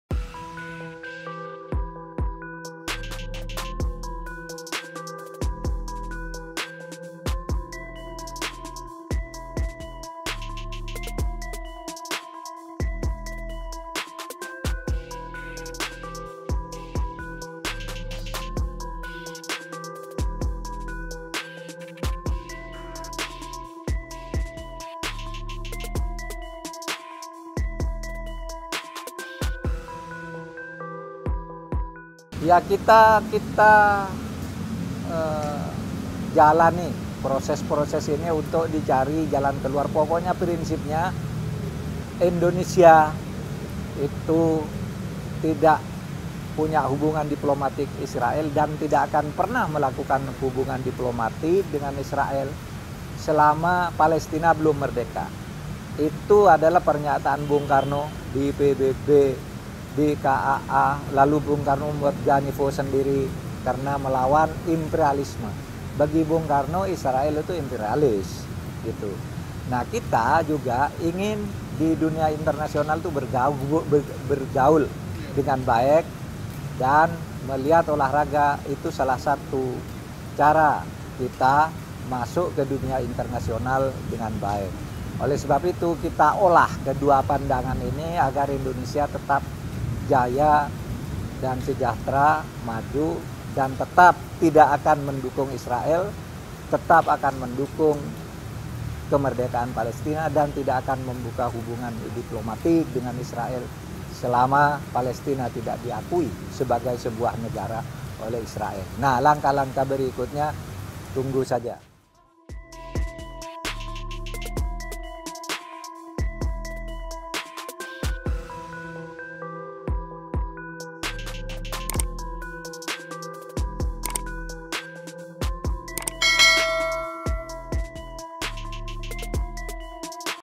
Ini Syarat Indonesia untuk Jalin Hubungan dengan Israel
KOMPAS.com - Menteri Koordinator bidang Politik, Hukum, dan Keamanan (Menko Polhukam) RI Mahfud MD memastikan pemerintah akan mencari jalan keluar mengenai polemik keikutsertaan Timnas Israel dalam Piala Dunia U-20 yang akan digelar di Indonesia.

Menurut dia, olahraga menjadi salah satu pintu masuk bagi Indonesia untuk bergaul di dunia internasional.

Hal ini disampaikan Mahfud di Kompleks Istana Kepresidenan, Jakarta pada Senin (27/03/2023).

Meski demikian, ia menegaskan bahwa Indonesia tidak memiliki dan tidak akan melakukan hubungan diplomatik dengan Israel selama Palestina belum merdeka.

Simak informasinya dalam video berikut.

Penulis: Ardito Ramadhan
Penulis Naskah: Anasthasya
Video Editor: Anasthasya
Produser: Adesari Aviningtyas